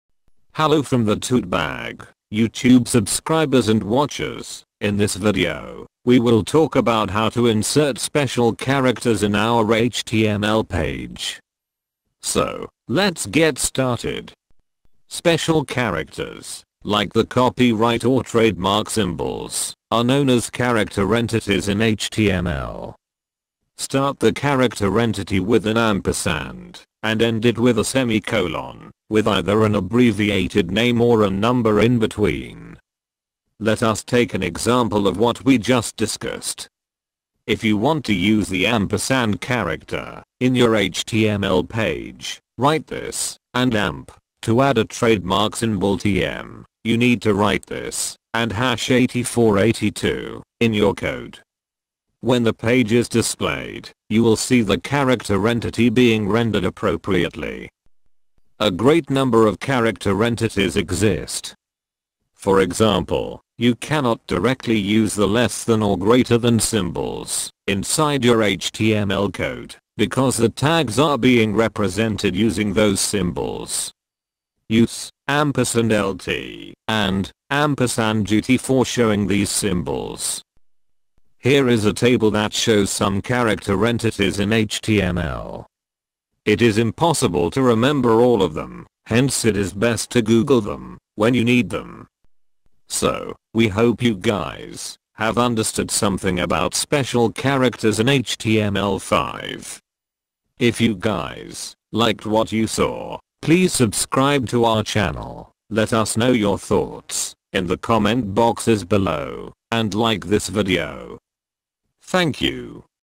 No 7: HTML Character Entities: HTML5 Tutorial For Beginners [HD 1080p]
This HTML5 Tutorial video talks about the HTML character entities. Character entities are a table of special symbols which are associated with either an abbreviated short name or a numeric code. You cannot directly insert certain symbols in your HTML page, hence character entities are the best way to use such symbols